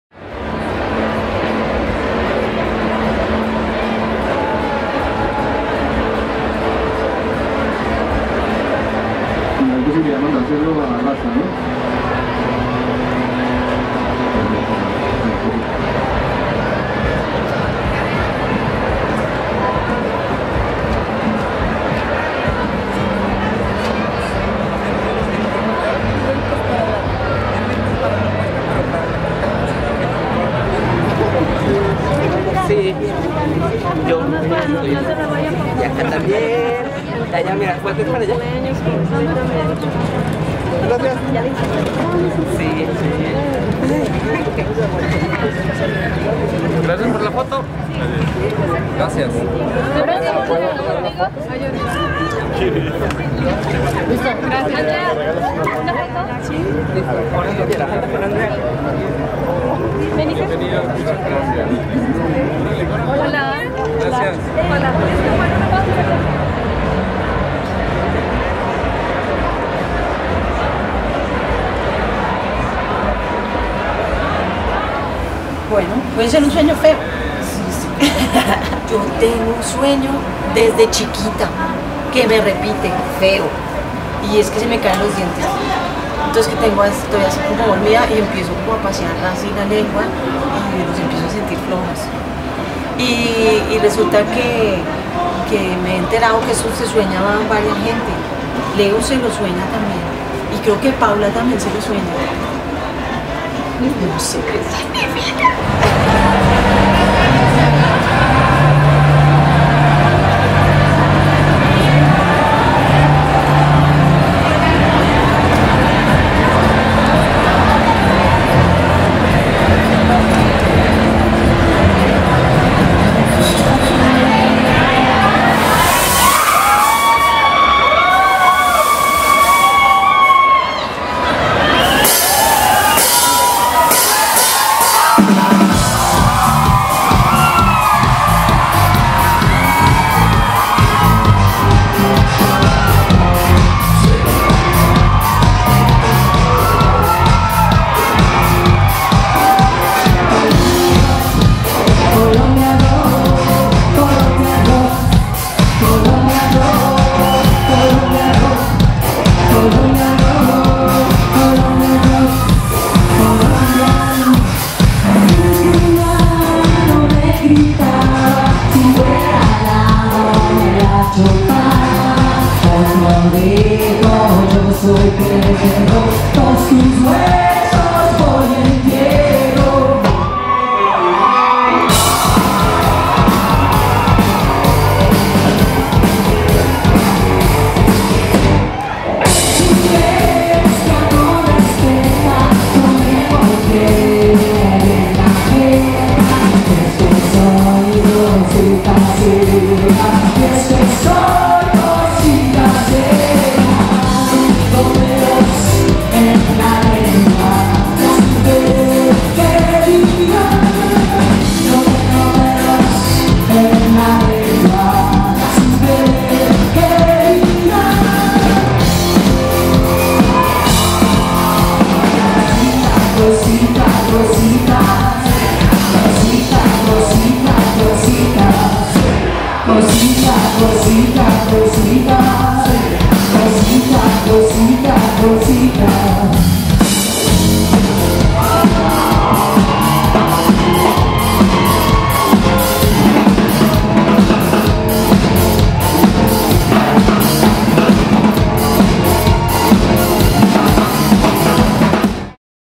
Aterciopelados en el Foro del Lago FNSM2017
Parte del "detrás del escenario" que se vivió previo y al comienzo del concierto que el grupo colombiano ofreció en el foro del Lago de la ciudad de Aguascalientes el día martes 18 de abril de 2017.